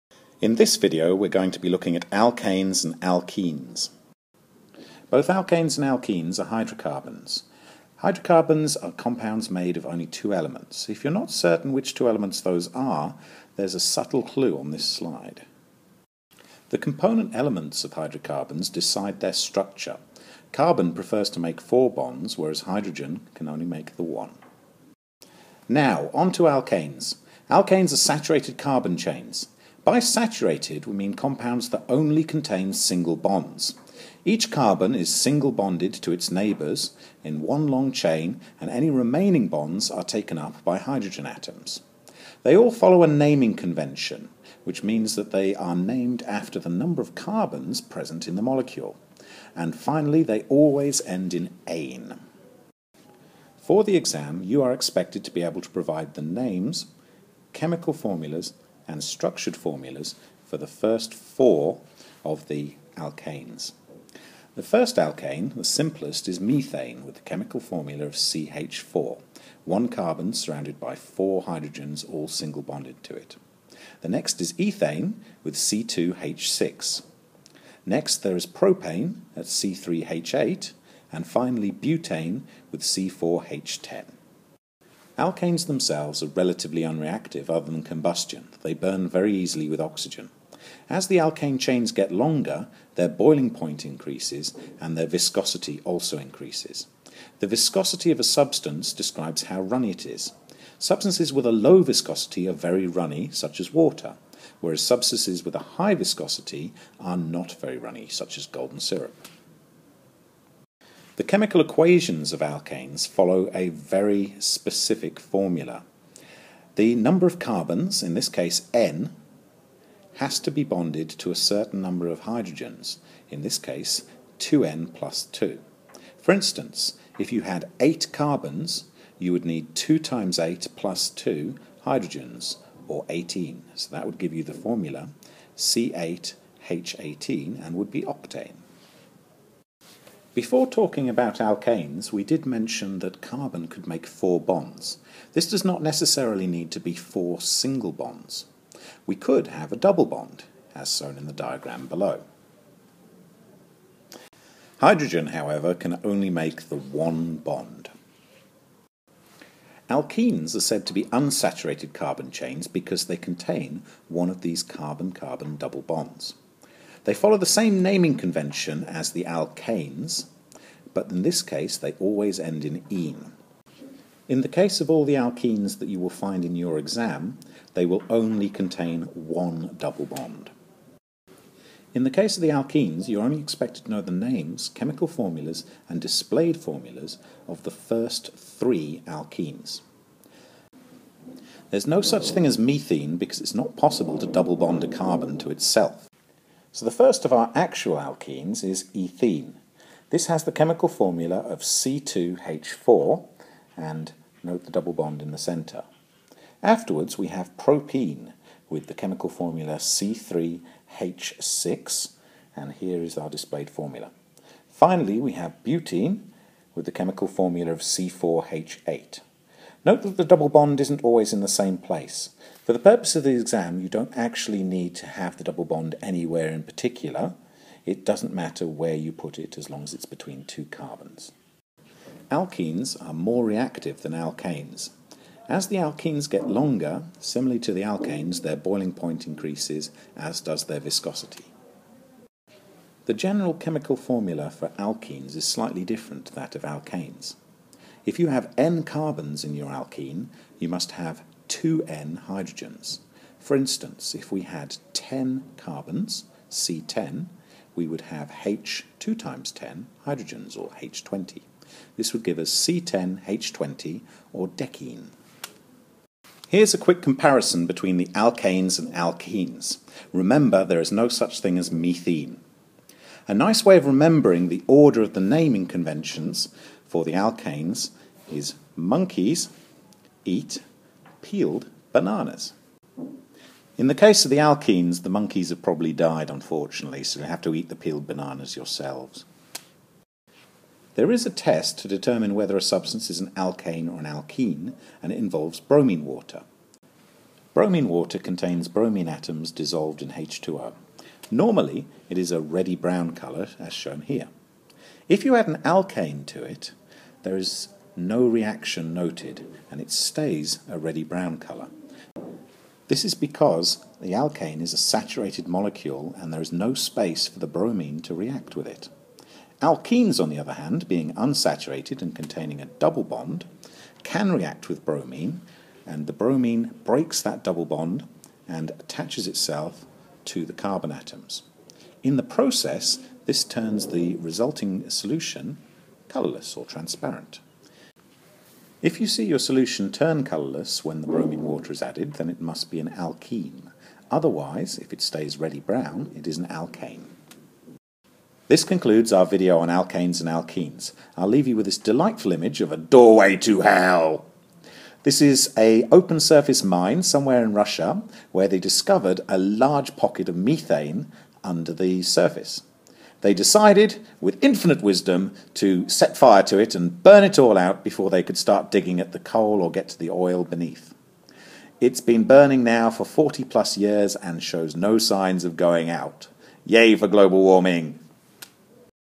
Alkanes & Alkenes
A short revision video reviewing the Edexcel Chemistry C1 topic of Alkanes & Alkenes. A first attempt, so please let me know what I need to improve!